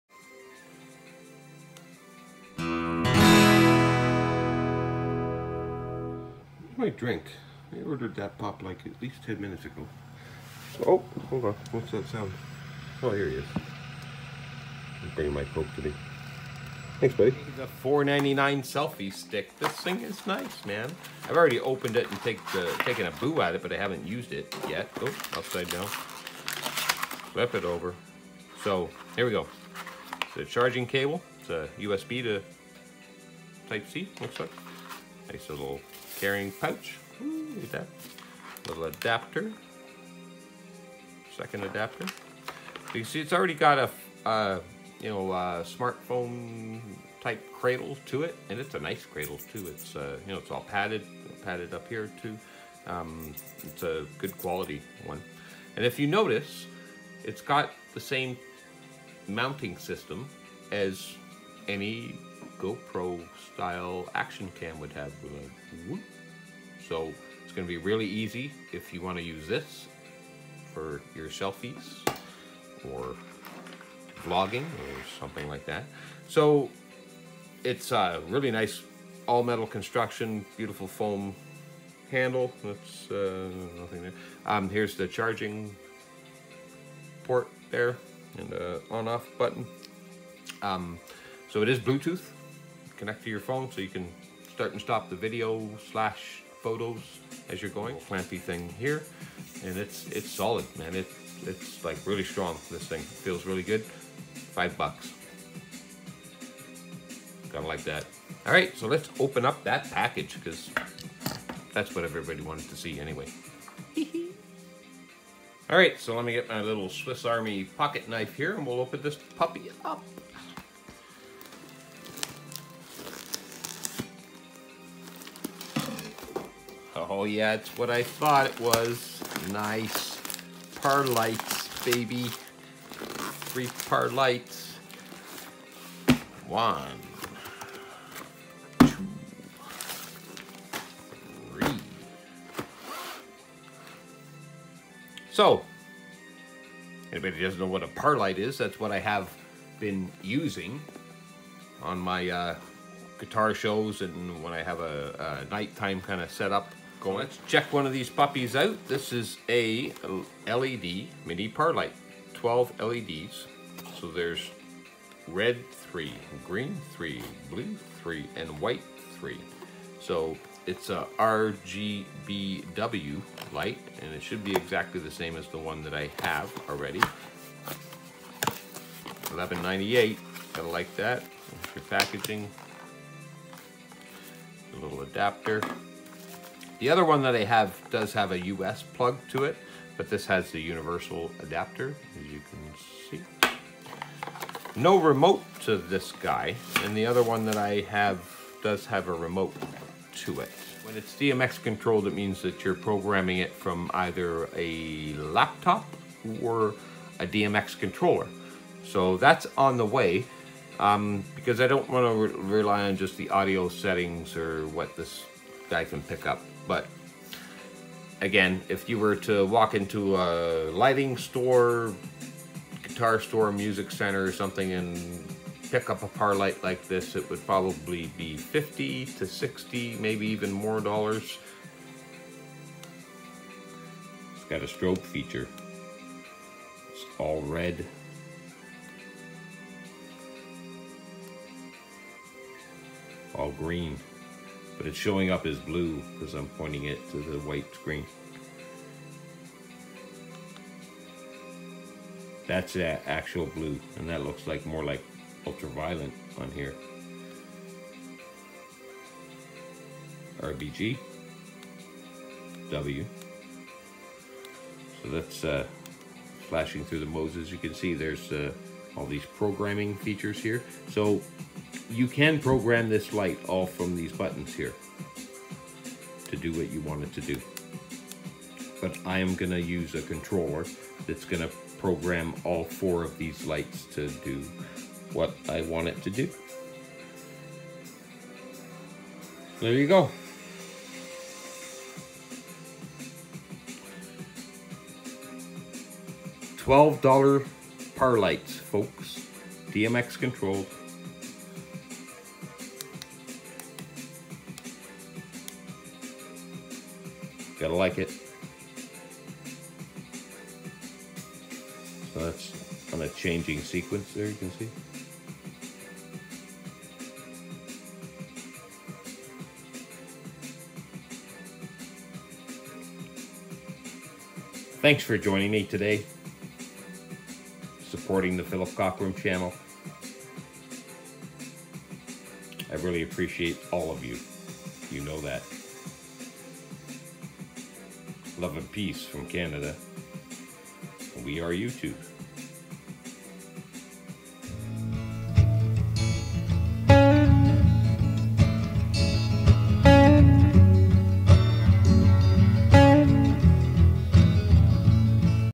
LED Mini Par Lights Unboxing and Reviewing
LED Mini Par Lights Unboxing and Reviewing .Par lights are used as stage lighting in many applications . And can be controlled with a computer, a DMX controller or you can program the light directly. But here is a more detailed description for those interested . Love and Peace from Canada
We are Youtube. You and Me
A parabolic aluminized reflector lamp (also PARCAN light, PARcan, or simply PAR) is a type of electric lamp that is widely used in commercial, residential, and transportation illumination. Usage includes locomotive headlamps, aircraft landing lights, and residential and commercial recessed lights ("cans" in the United States). They are identical in principle to sealed beam automobile headlights.

The lamps and their fixtures are widely used in theatre, concerts and motion picture production when a substantial amount of flat lighting is required for a scene.

In situations where sunlight or other specular light is available, a white foam reflector is often used to accomplish the same effect as a PAR array.

PAR cans are being replaced in some stage applications by LED PAR cans, which use less electric power and produce a wide array of saturated colors without the use of color filters, when white light is not needed.